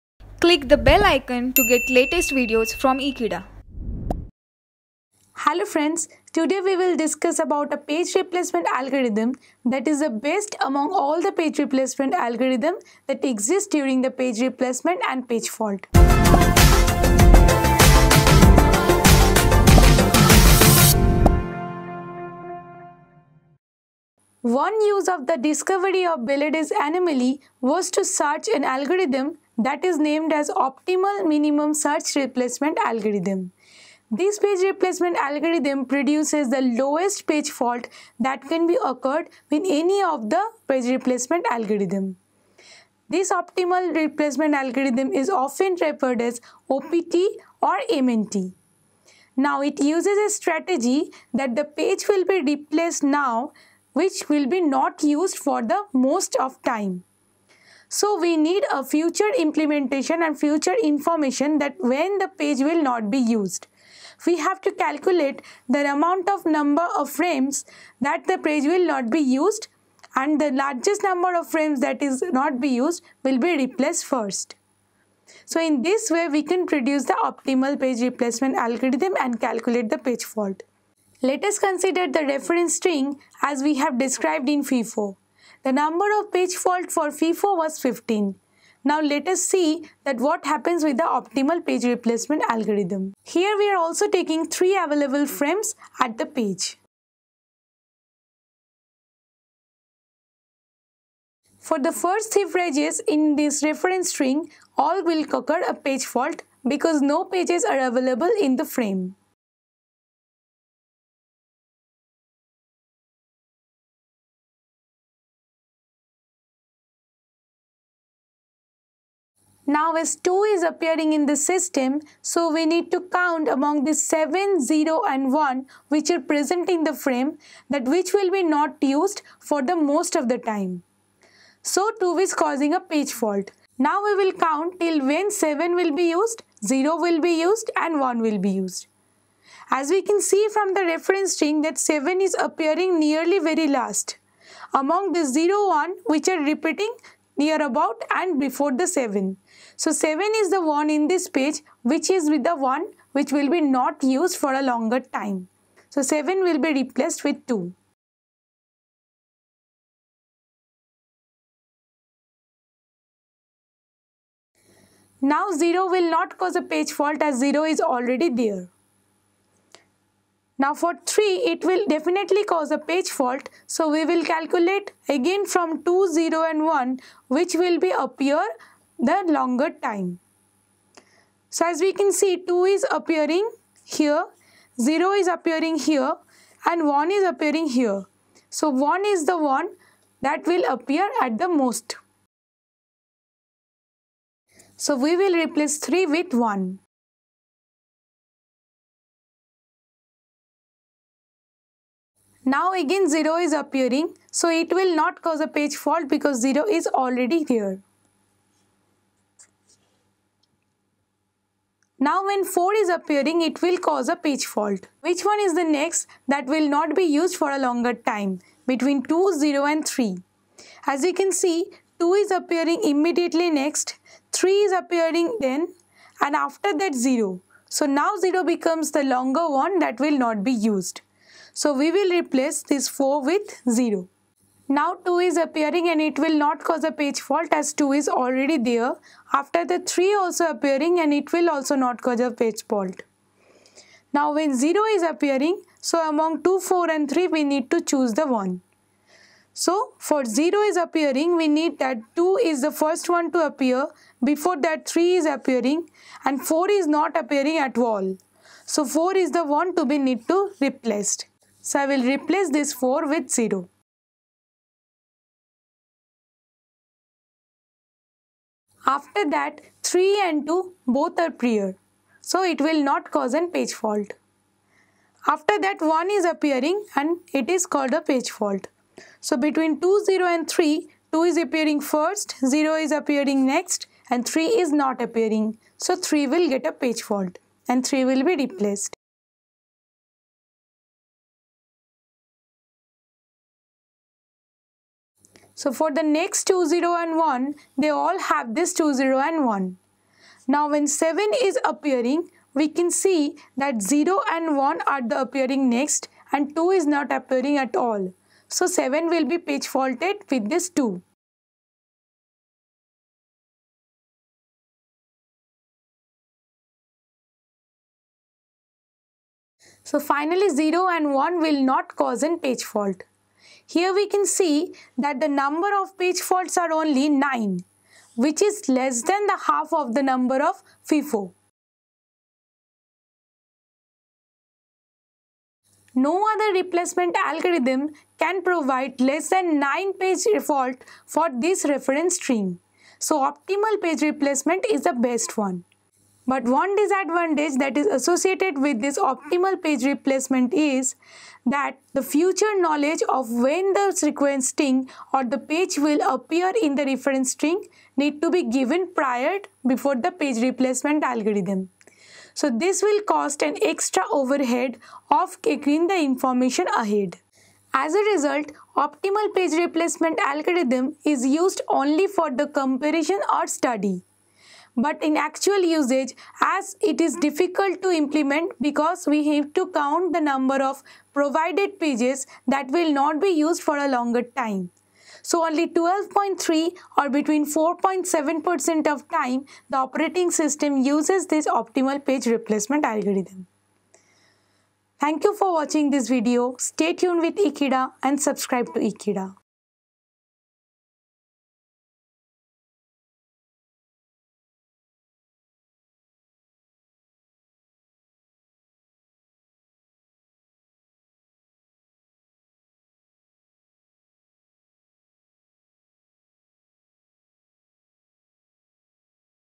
Optimal Page Replacement - Memory Management - Operating System
Optimal Page Replacement Video Lecture from Memory Management Chapter of Operating System Subject for all engineering students.

Happy Learning : )